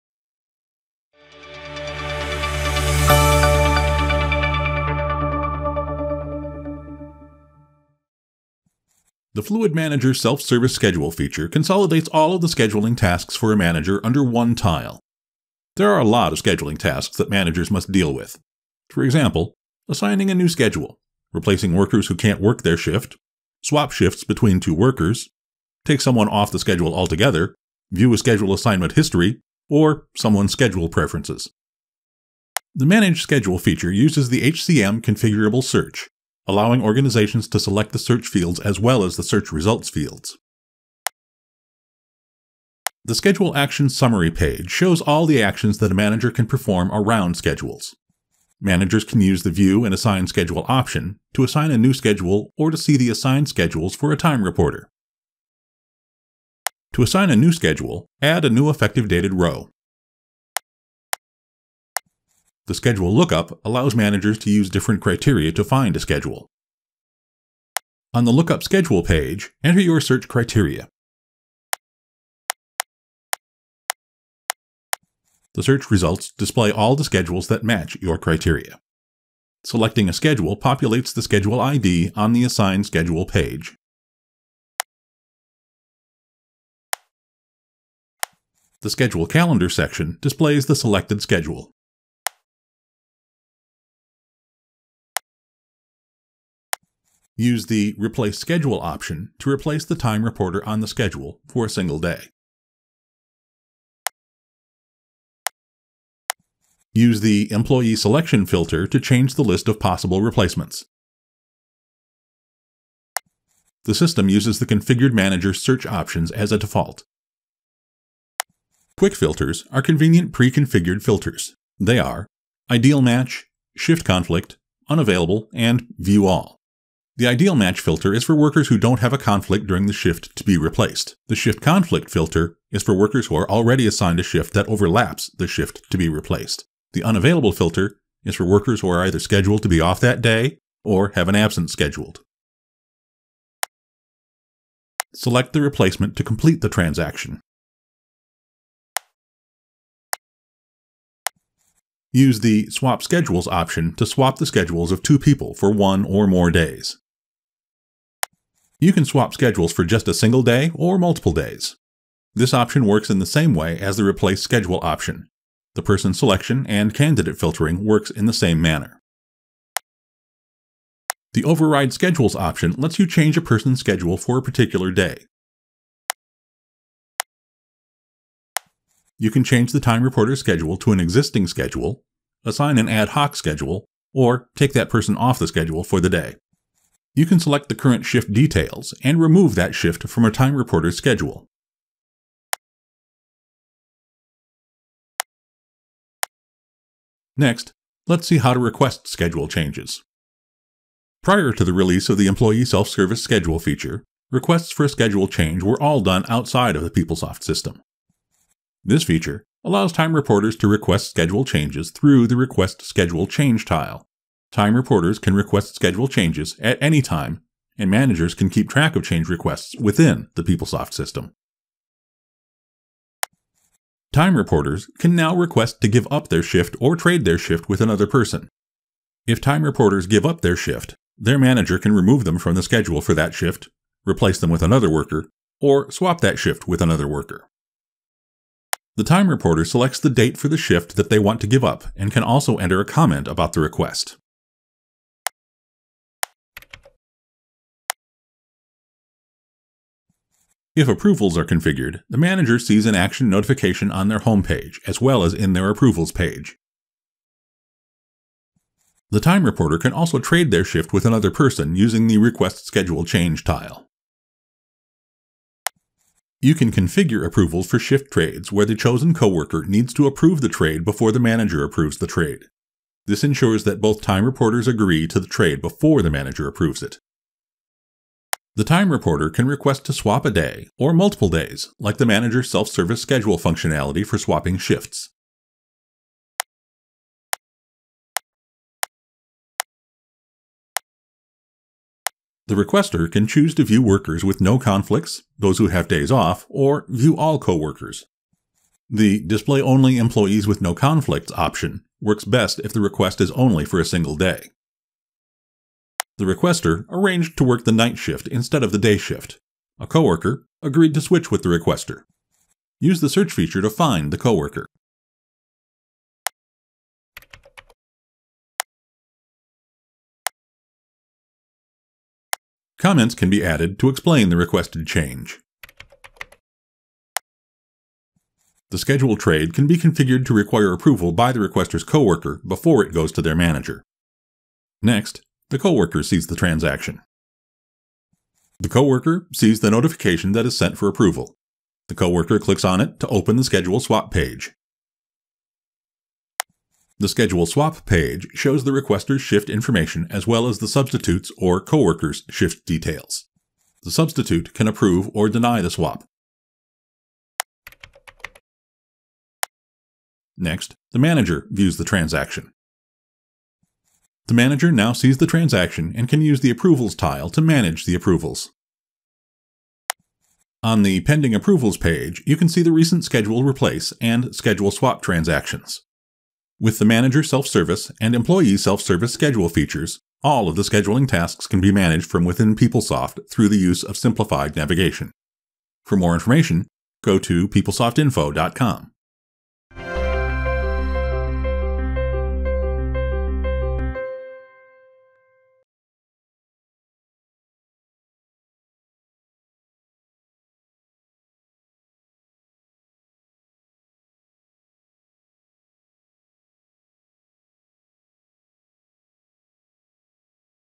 PeopleSoft Manager and Employee Self Service Schedules
The Fluid Manager Self-Service Schedule feature consolidates all the scheduling tasks for a manager under one tile.

There are a lot of scheduling tasks that managers must deal with. For example, assigning a new schedule, replacing workers who can’t work their shift, swap shifts between two workers, take someone off the schedule altogether, view schedule assignment history or someone’s schedule preferences.